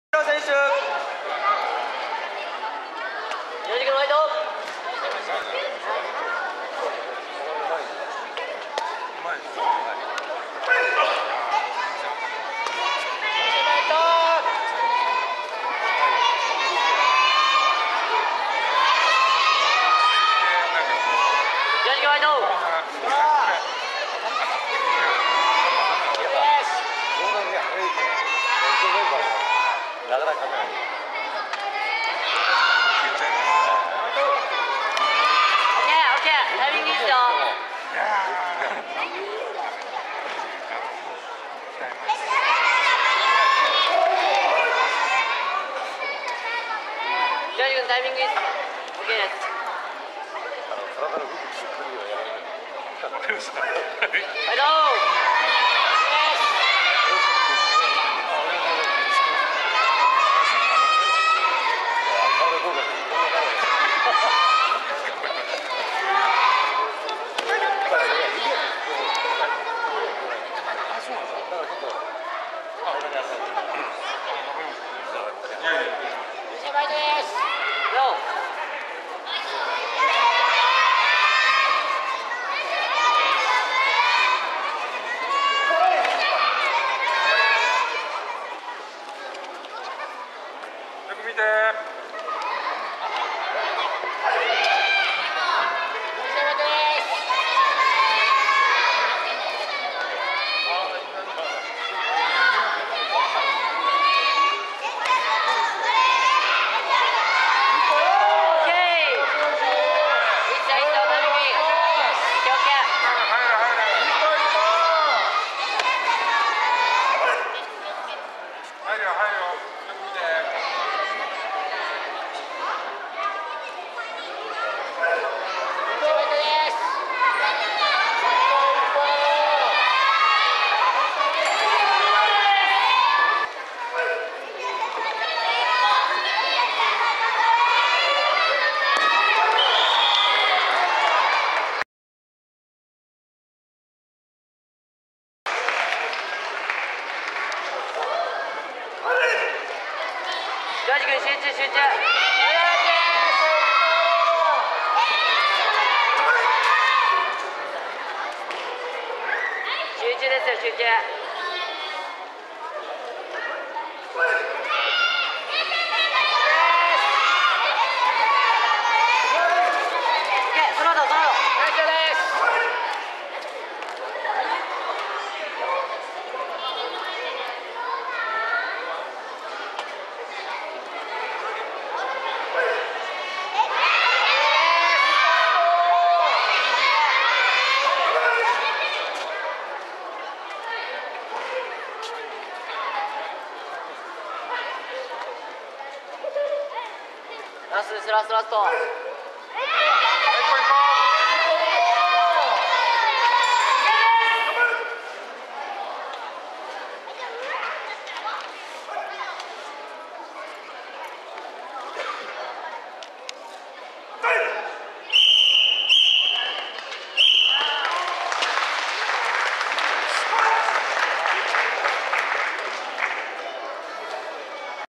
2013 東京都防具付空手道選手権大会 準決勝第二試合
2013年　東京都防具付空手道選手権大会
赤　吉田拓也（忍誠館草加）
白　菊村城次（錬武会厚木）
主催　全日本空手道連盟 錬武会 東京都本部

長らく防具付き空手の全国の頂点から離れている東京勢の大会。
準決勝第二試合は両者とも他県の選手。
柔らかいステップと伸びのある突きを武器に勝ち上がってきた菊村選手に対し、上段を引き付けつつ中段蹴りを決めた、吉田選手が技あり1-0で勝利しました。

JKF RENBUKAI
Tokyo bogu karate championship 2013
Joji Kikumura 0 - 1 Takuya Yoshida